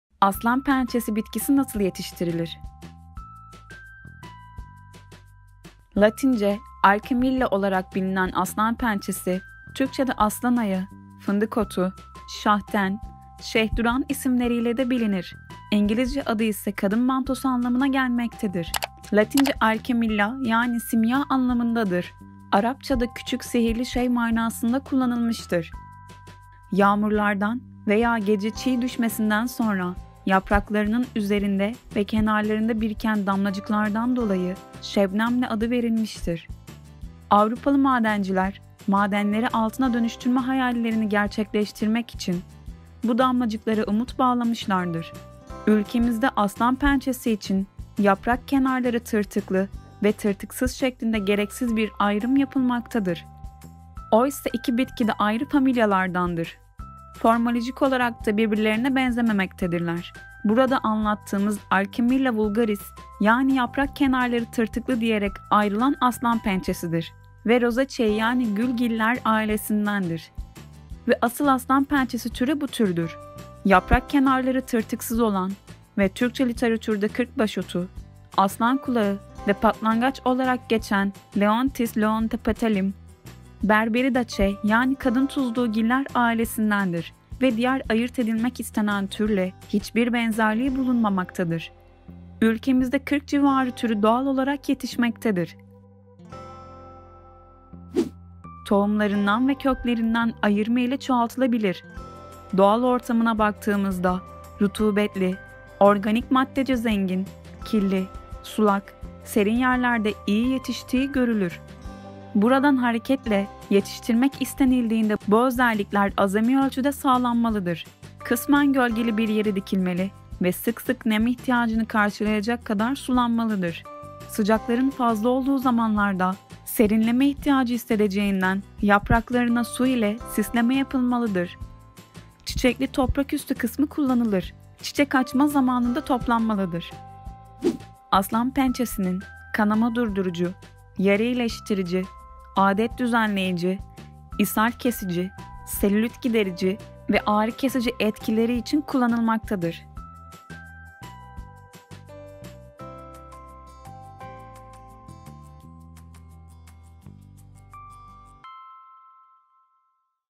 Tıbbi ve Aromatik Bitkiler Serisi: ASLANPENÇESİ Nasıl Yetiştirilir? İşte Kullanım ve Faydaları!
Tıbbi ve Aromatik Bitki Serisi: ASLANPENÇESİ Nasıl Yetiştirilir?

Türkçede "aslanayağı, fındık otu, şahten, şehduran" isimleriyle de bilinir. İngilizce adı ise "kadın mantosu" anlamına gelmektedir. Latince Alchemilla yani "simya" anlamındadır. Arapçada "küçük sihirli şey" manasında kullanılmıştır.

Yağmurlardan veya gece çiy düşmesinden sonra yapraklarının üzerinde ve kenarlarında biriken damlacıklardan dolayı “şebnemli” adı verilmiştir. Avrupalı madenciler, madenleri altına dönüştürme hayallerini gerçekleştirmek için bu damlacıklara umut bağlamışlardır.

Peki bu bitkinin toprak şartları nelerdir? Hangi iklim şartlarında yetişir? Sulama ve hasat ne zaman yapılmalıdır? Aslanpençesi kullanım alanları nelerdir?  Aslanpençesi faydaları nelerdir?